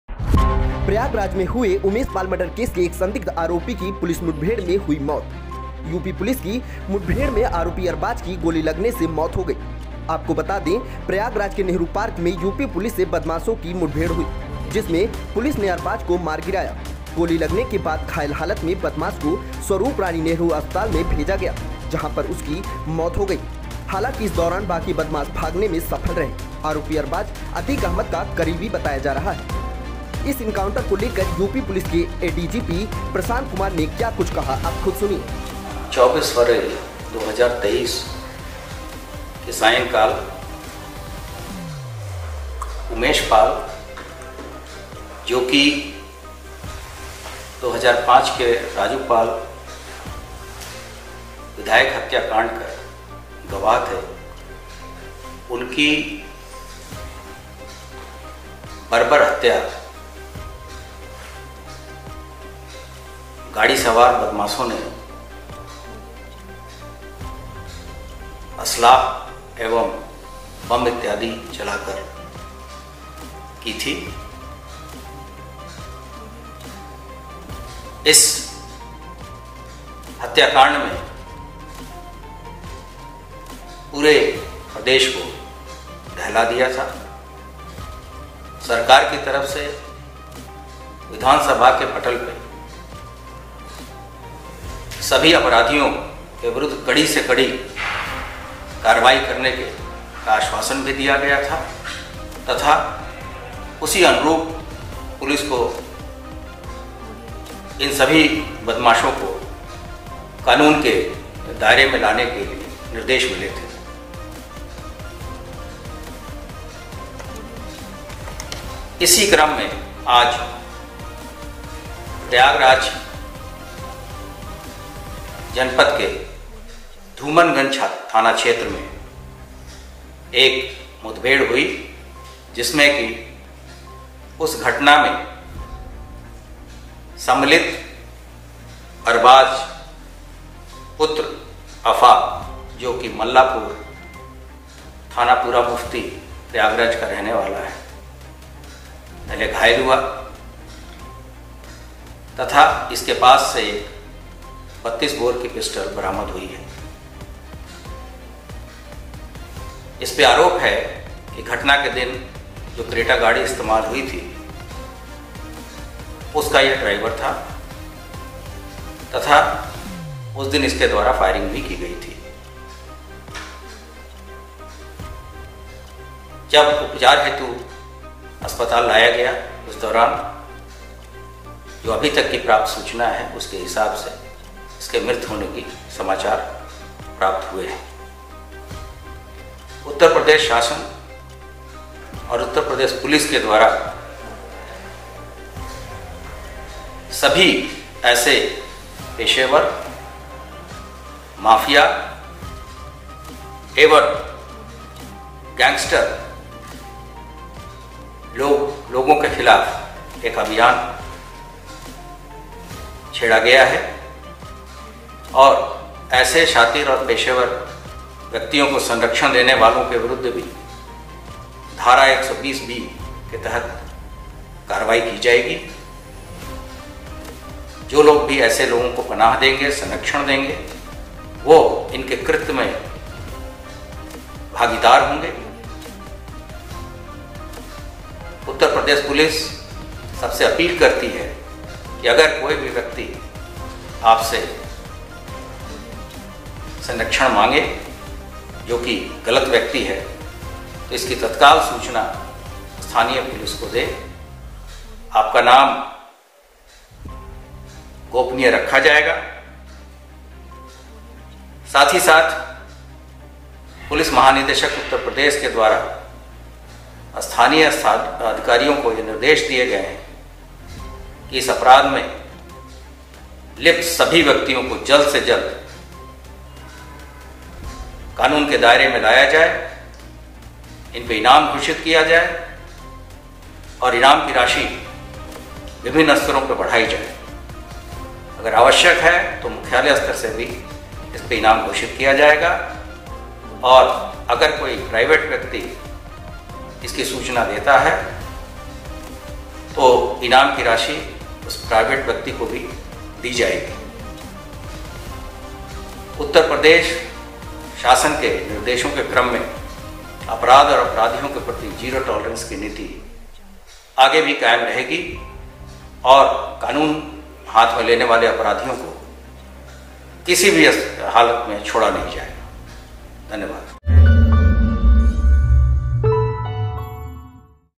प्रयागराज में हुए उमेश पाल मर्डर केस के एक आरोपी अरबाज की पुलिस मुठभेड़ हुई मौत
प्रयागराज में हुए उमेश पाल मर्डर केस के एक संदिग्ध आरोपी की पुलिस मुठभेड़ हुई मौत. यूपी पुलिस की मुठभेड़ में आरोपी अरबाज की गोली लगने से मौत हो गई. आपको बता दे प्रयागराज के नेहरू पार्क में यूपी पुलिस से बदमाशों की मुठभेड़ हुई, जिसेमें पुलिस ने अरबाज को मार गिराया. गोली लगने के बाद घायल हालत में बदमाश को स्वरूपरानी नेहरू अस्पताल में भेजा गया, जहां पर उसकी मौत हो गई. हालांकि इस दौरान बाकी बदमाश भागने में सफल रहे, आरोपी अरबाज अतीक अहमद का करीबी बताया जा रहा है.

इस एनकाउंटर को लेकर यूपी पुलिस के ADGP प्रशांत कुमार ने क्या कुछ  कहा आप खुद सुनिए,,